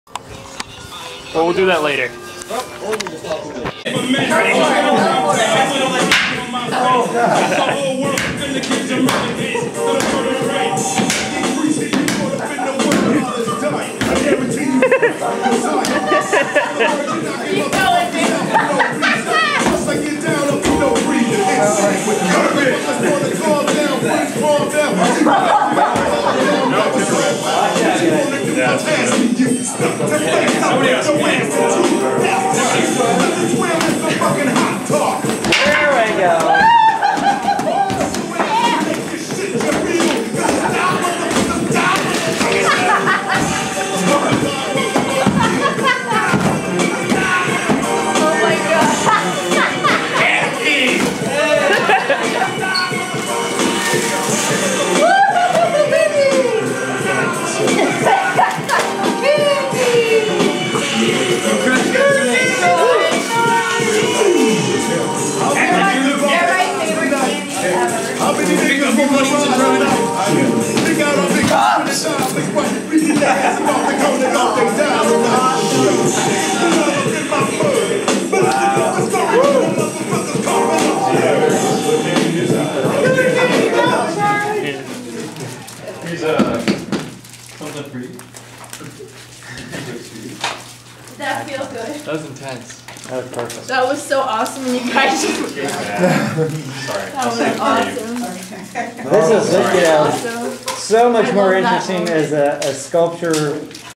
human pinata
This is a plaster mold of my body for my sculpture class. I originally had it suspended, however the twine snapped and landed face first, which made it even more interesting. I asked my class to smash it. They didn't know there was candy in there.